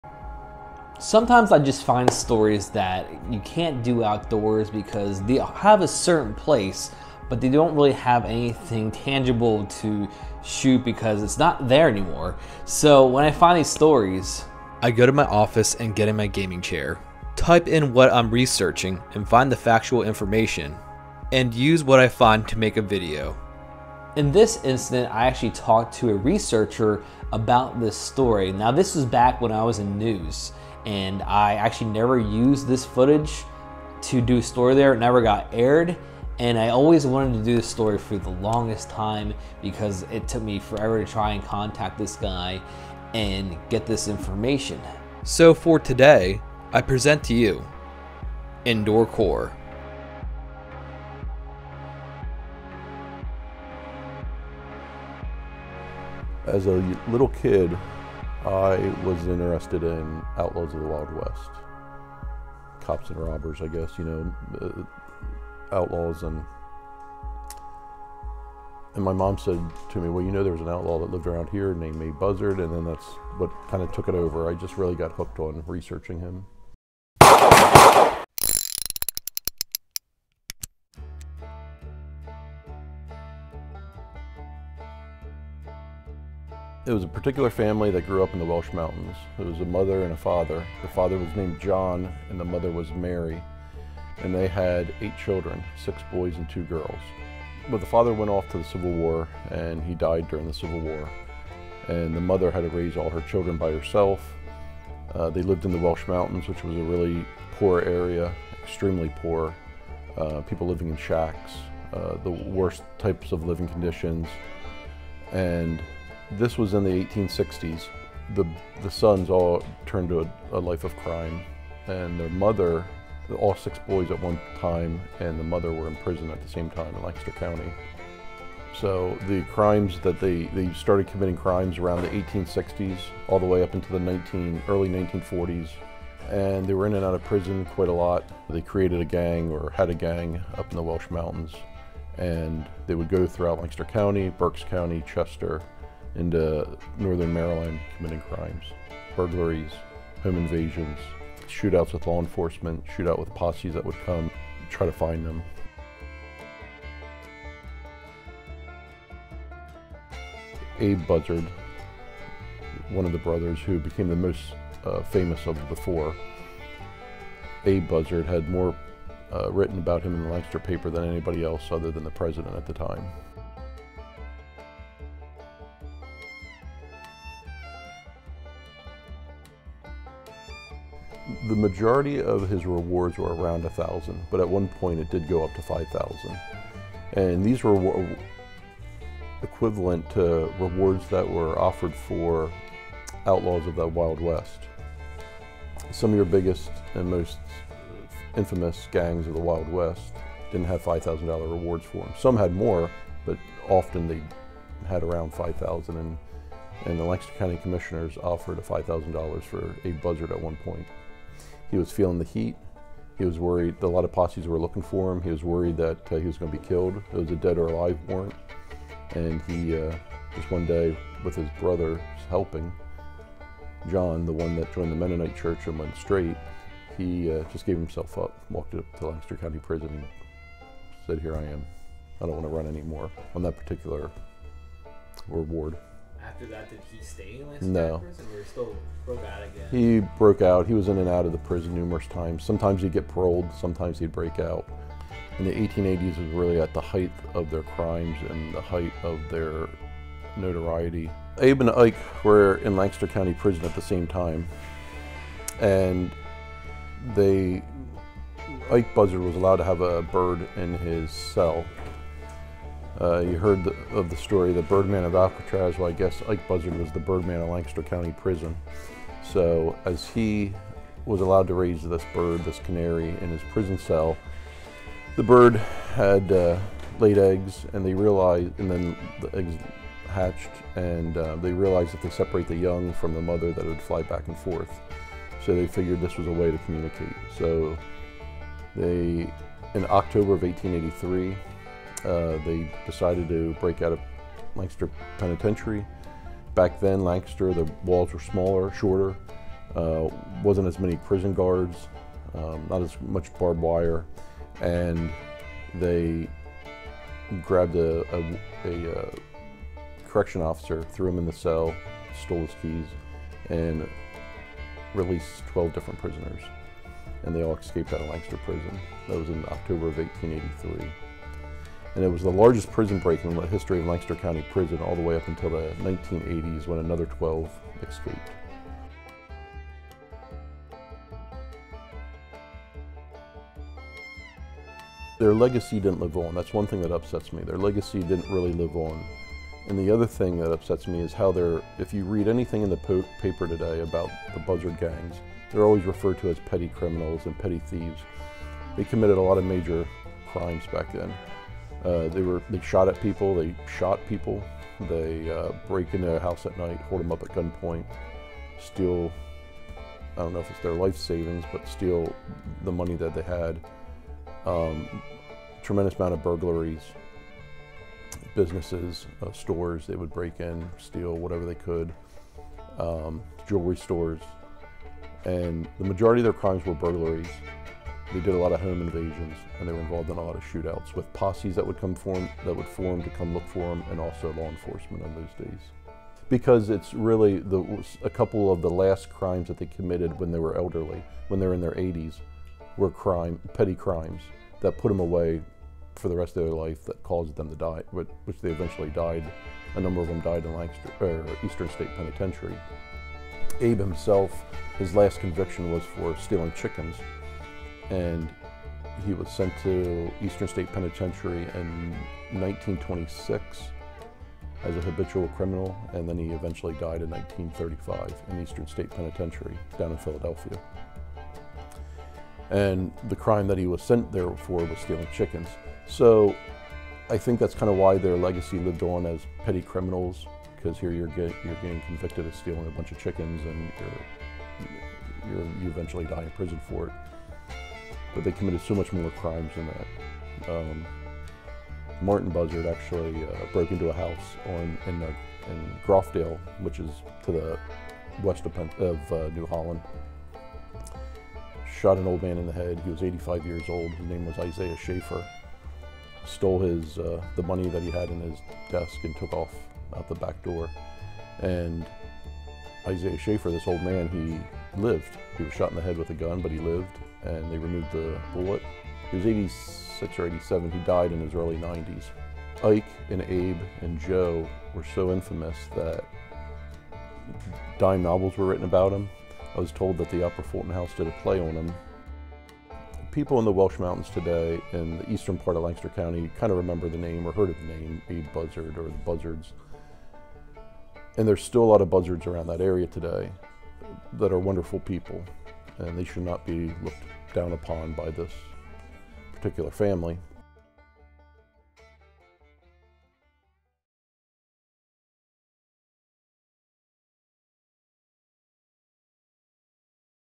THE WILD EAST: THE BUZZARD GANG
I talk to a local expert about a notorious outlaw gang in the 1800's.

History channel

History of the United States

Interesting history stories

History and the concept of time